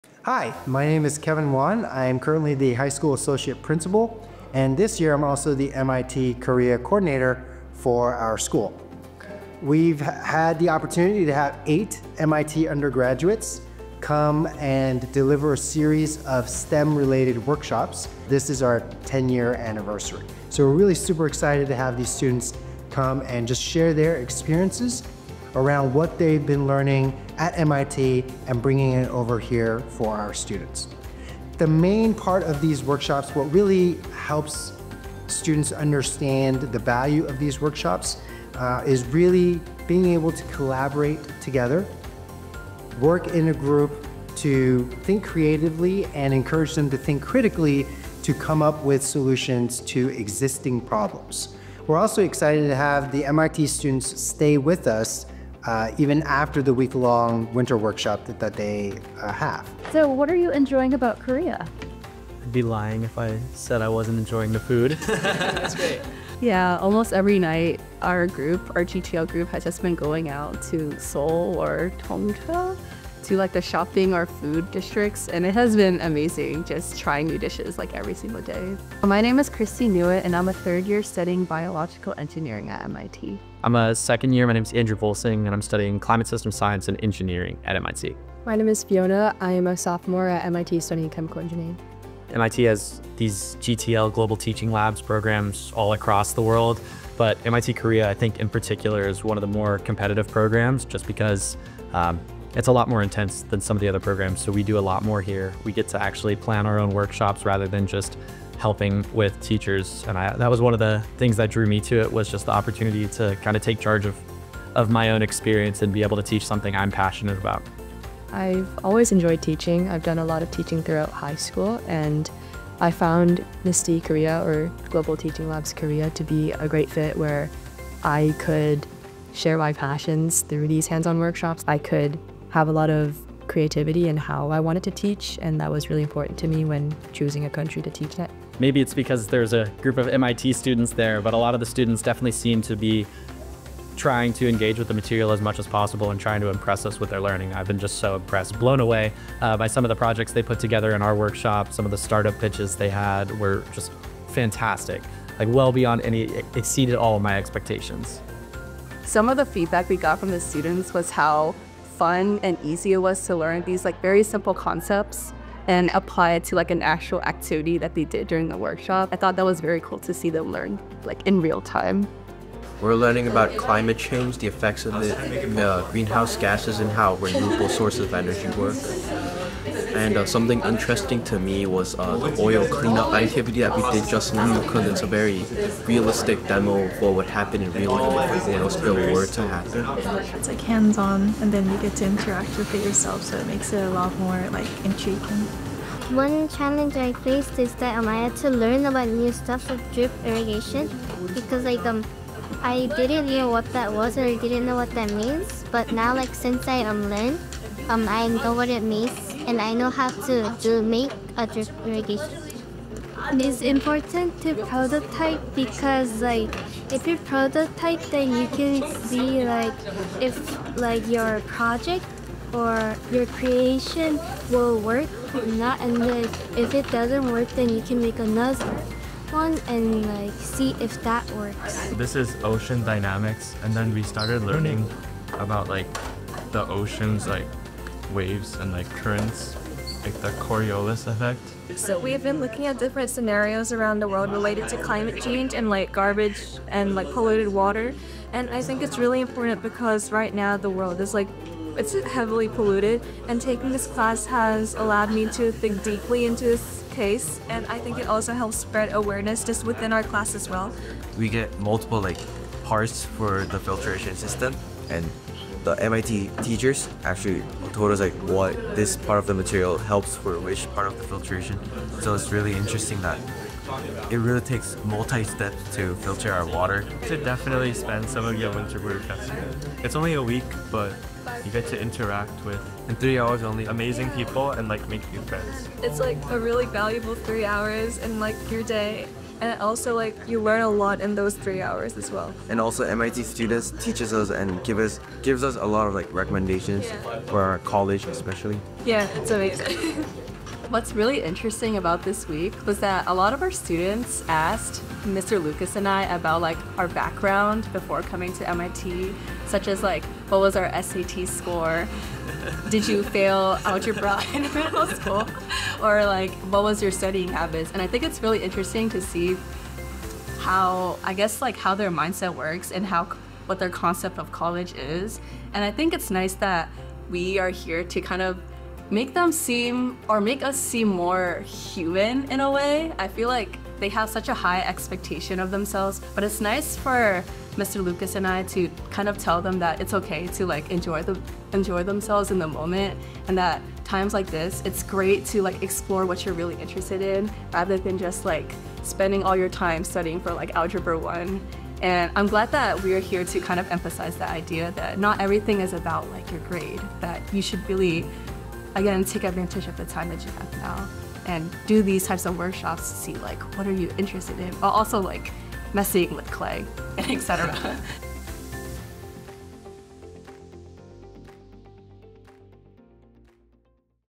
Community Voices: MIT Korea Winter Workshops
MIT students from the MIT Global Teaching Labs are on campus this month! High school associate principal Dr. Kevin Wan shares more about the MIT-Korea program and the opportunities this collaboration provides our students. KIS and MIT students also tell about their learning and teaching experiences.